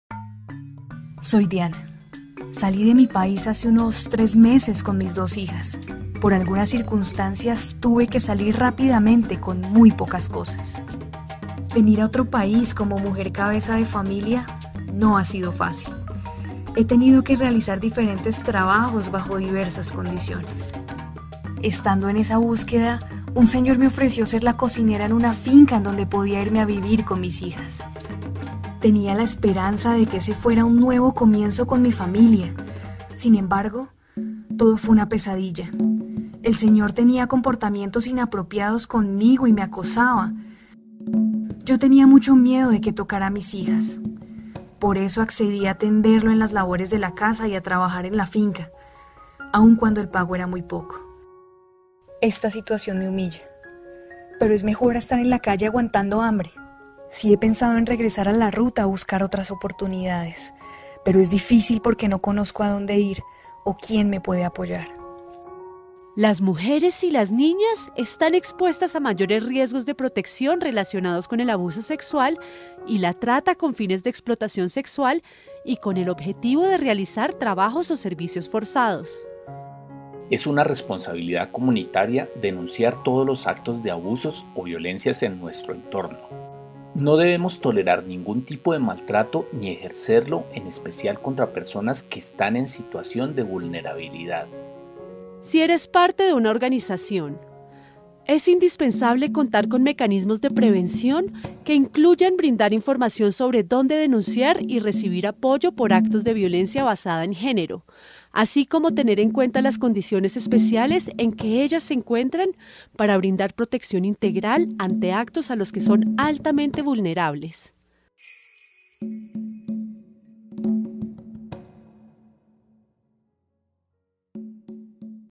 Violencia diferencial
Las mujeres cabezas de familia, refugiadas, desplazadas y migrantes y los abusos a los que son sometidas.
Clip realizado para la IFRC Federación Internacional de la Cruz Roja